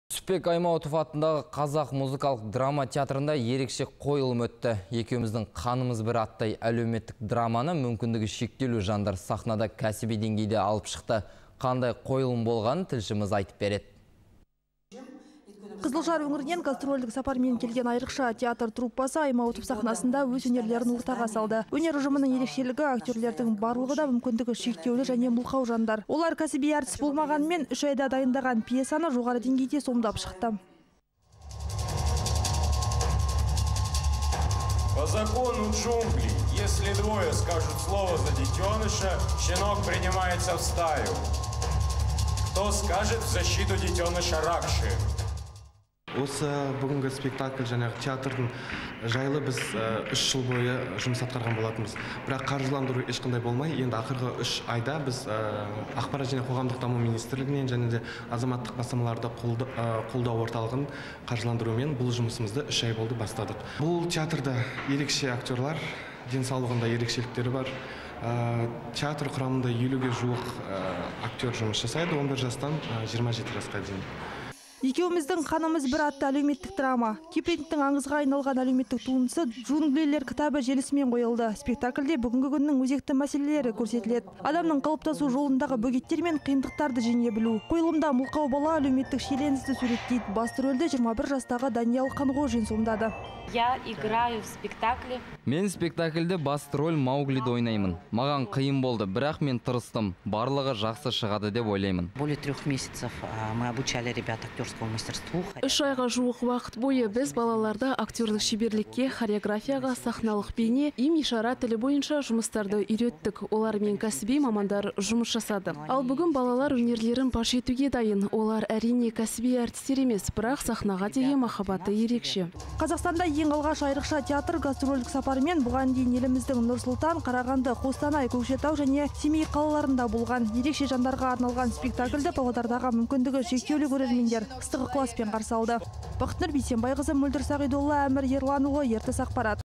ЕРЕКШЕ ҚОЙЫЛЫМ
Жүсіпбек Аймауытов атындағы қазақ музыкалық-драма театрында ерекше қойылым өтті.